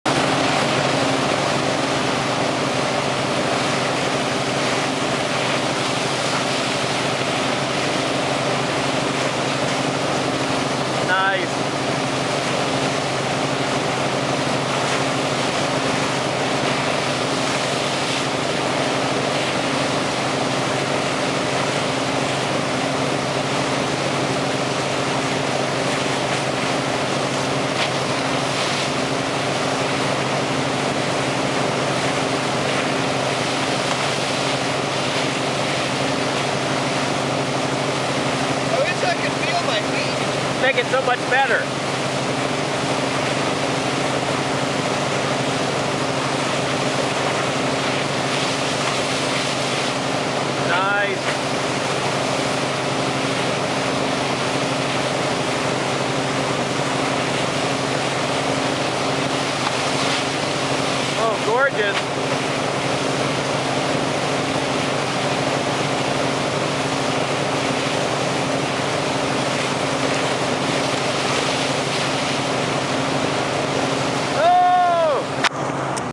James Walker wakesurfing the new concave deck Flyboy Wakesurf prototype
So it's been ridiculously cold here in NorCal, but we were able to take the new Flyboy Wakesurf concave deck wakesurf board prototype out for a quick test.  This short unedited clip shows James Walker wakesurfing it and landing: stalled ollie 3, slash, bottom turn, air 180, air 3, surface reverse, air reverse almost.

The shape seems to be on track, now if we can just get some warmer weather to give it a thorough test!  Be sure to visit the Flyboy Wakesurf blog for the write up.